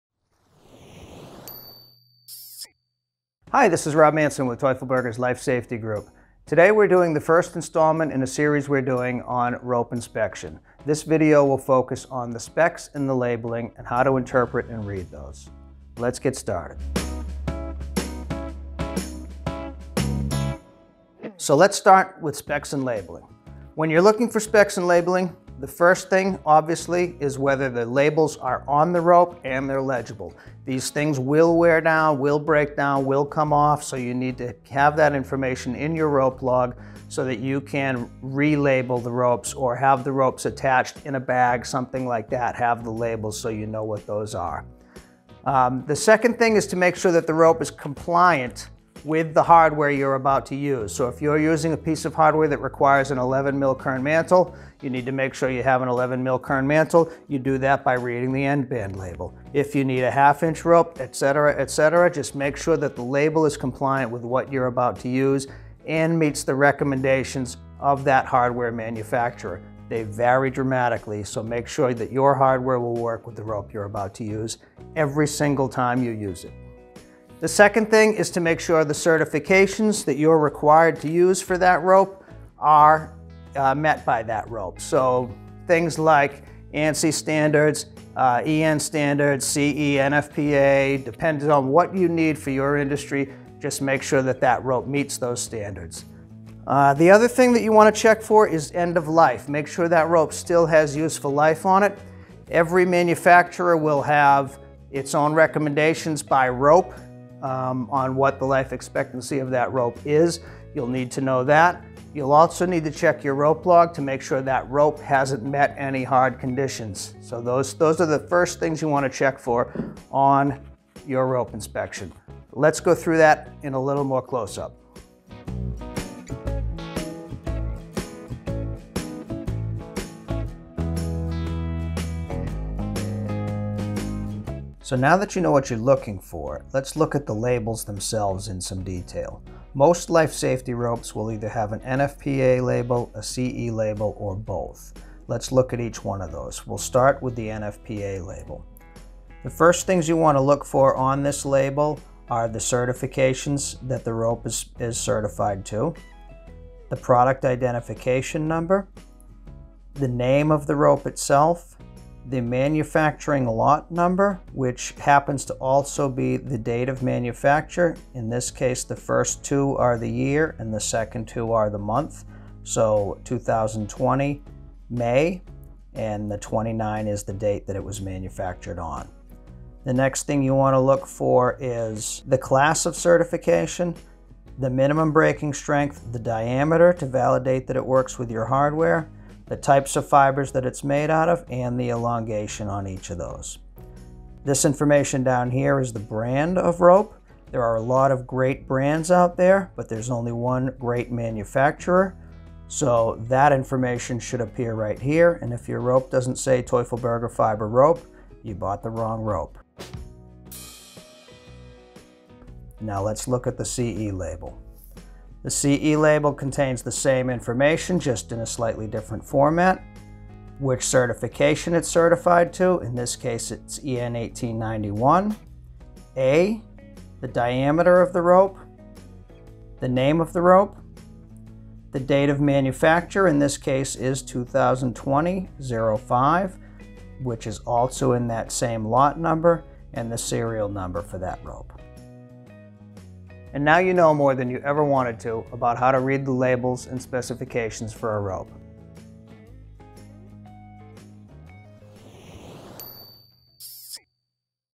Rope inspection Part 2 - Specs and Labelling
The labels of a rope contain a lot of data. In your rope inspection you should check this data carefully. In the video, our segment manager Rob Manson explains you the Teufelberger labelling and what you should pay attention to.
Referring to Rob there are many good rope brands but there is only one good manufacturer. :-)
If you want to know more about that - watch the video till the end.